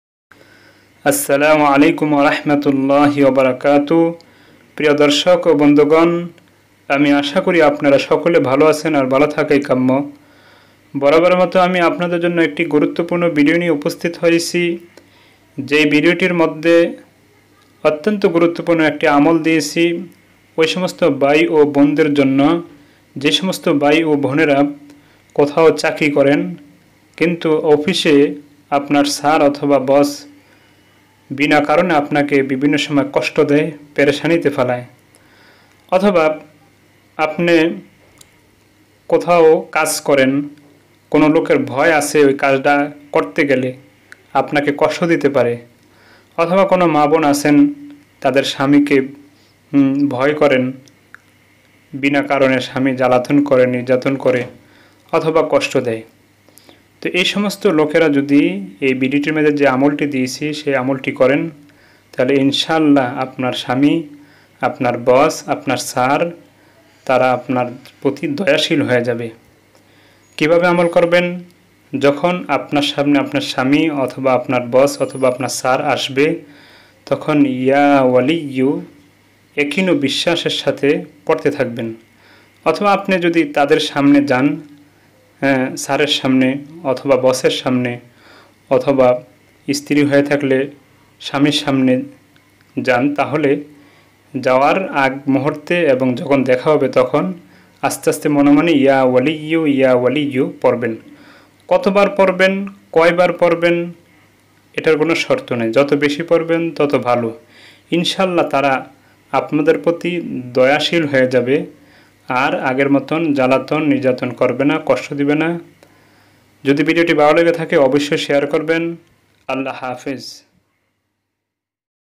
কেউ আপনাকে পেরেশানি করলে এই আমলটি করবেন আপনার উপর দয়াশীল হয়ে যাবে। Islamic life
Assalamu alaikum!

মোবাইল: ০১৩১৬১২৬১১৩

কেউ যদি আপনাকে কষ্ট দেয়, পেরেশানি করে, জ্বালায়, তাহলে আমলটি নিয়ম অনুযায়ী করলে ভালো ফল পাবেন। এধরনের আচরণ থেকে বিরত থাকবে ইনশাআল্লাহ।